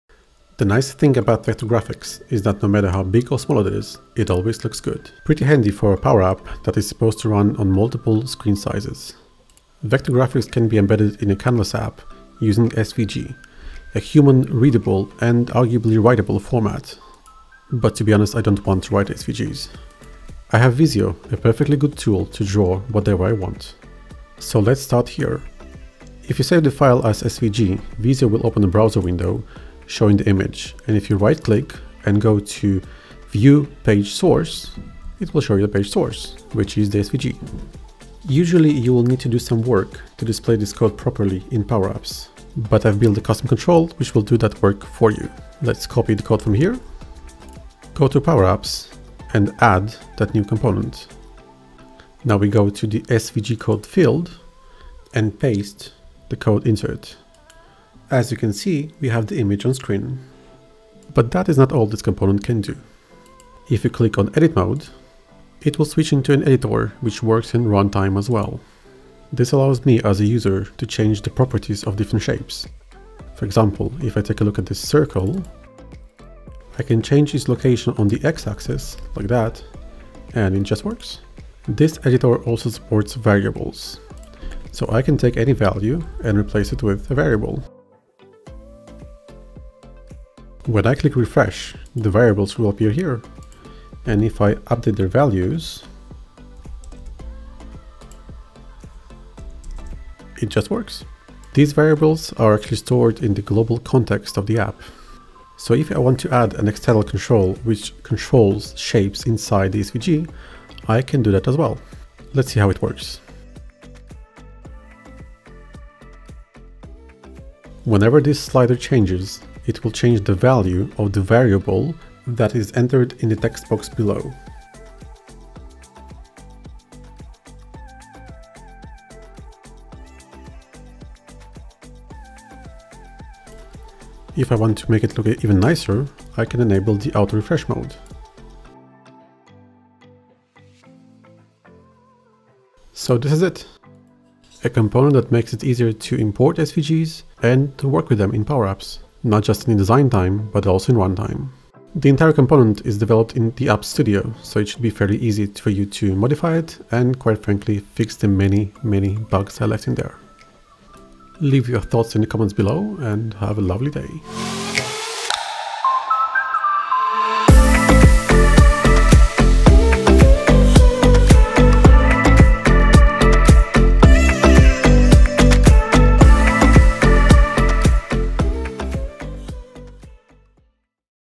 ⭐ NEW PowerApps control to manipulates scalable graphics (SVG)
I've built a PowerApps component that displays, edits and manipulates scalable graphics (SVG).